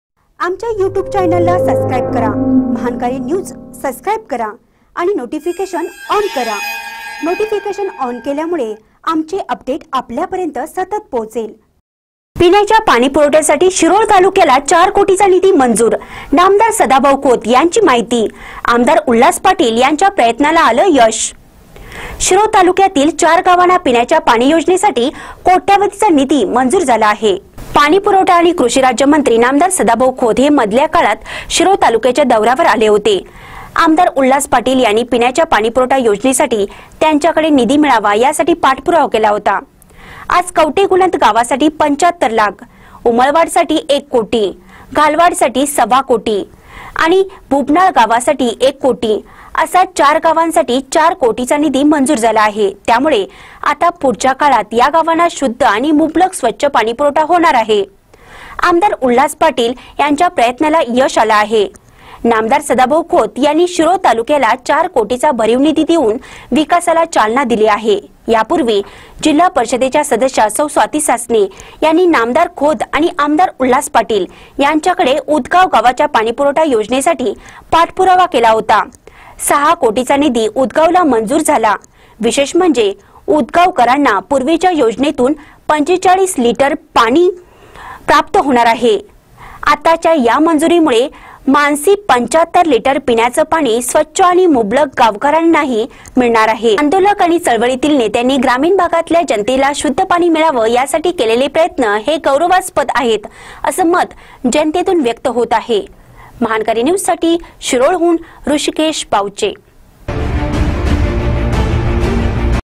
पिण्याच्या पाणी पुरवठ्यासाठी शिरोळ तालुक्यात चार कोटीचा निधी मंजूर
पिण्याच्या पाणी पुरवठ्यासाठी शिरोळ तालुक्यात चार कोटीचा निधी मंजूर : ना.सदाभाऊ खोत यांची माहिती : आ.उल्हास पाटील यांच्या प्रयत्नाला आलं यश.